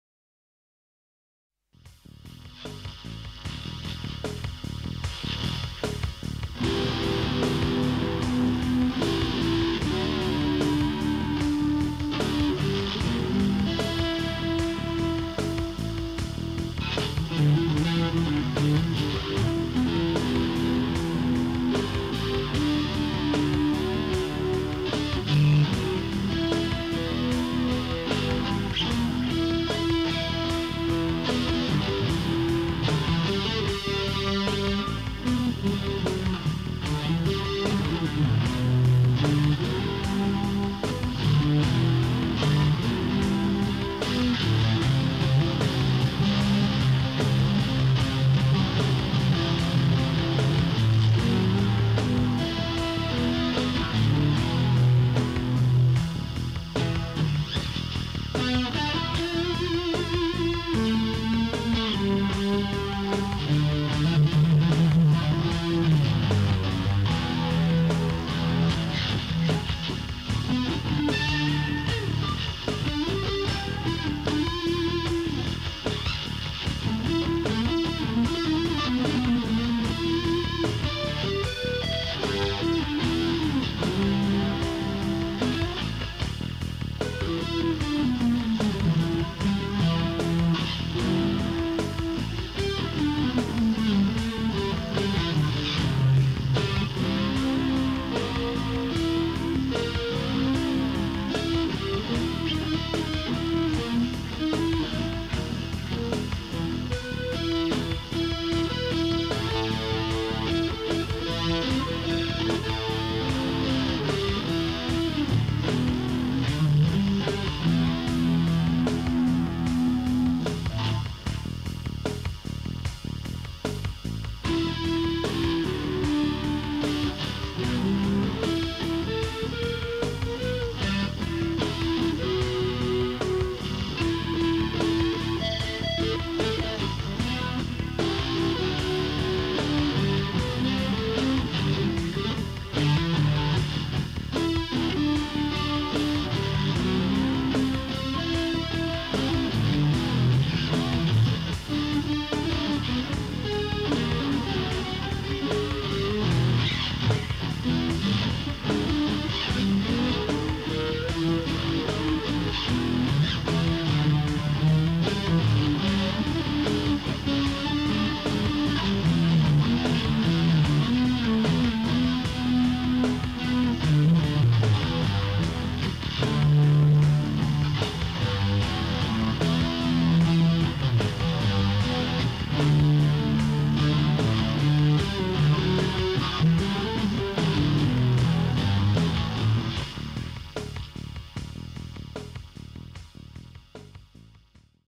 Rock10
Collage vidéo sur enregistrement d'une impro de janvier 2013.
Images de base tirées du film " Koyaanisqatsi " de G. Reggio (1982).